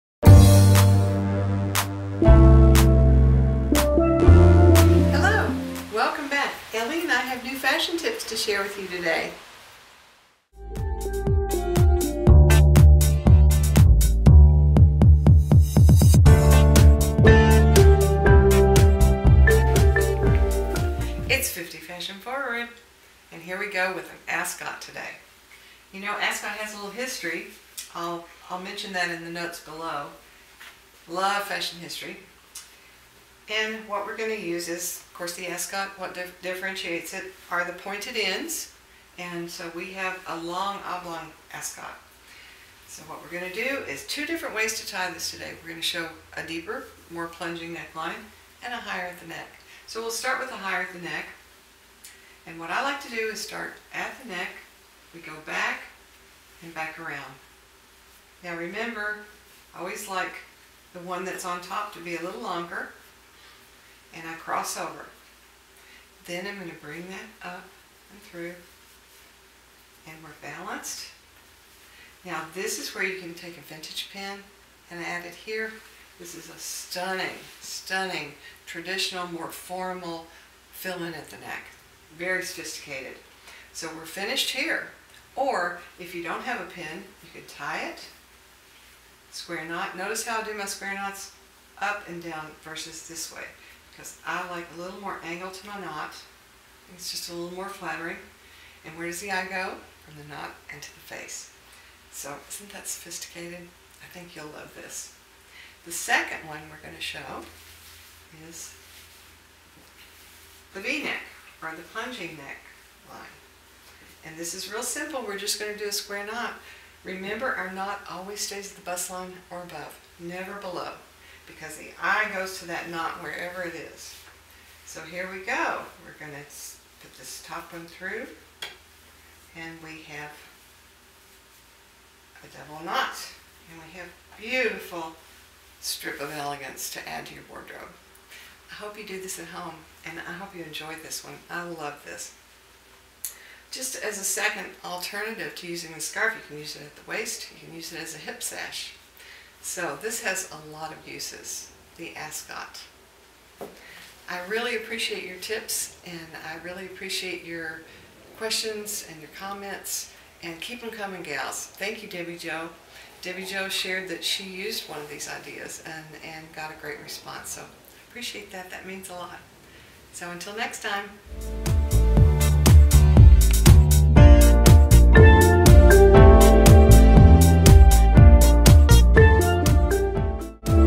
The ASCOT Scarf
Fashion History Note: The Ascot was first worn at the Ascot Heath races in the late 1800s. It replaced the cravat (original scarf). Although the original was gray with pointed ends, it was the beginning of the many variations in color and size of scarves today.

Since fashion is our greatest tool for personal expression, accessorizing with scarves can be an easy tool for you to differentiate your own personal style.

This scarf idea polishes and softens classic separates.

For those of you who like to cover the neck, it is perfectly understated. Using  silk charmeuse presents a statement of quality standards. Try it. This is a keeper!

Scarf dimensions: 3 1/2" x 68" long.

Special thanks to Bill Harris!

Reference material from: “Darling Where’s Your Wrap?” - Amazon.com - LINK: goo.gl/0Z5HBL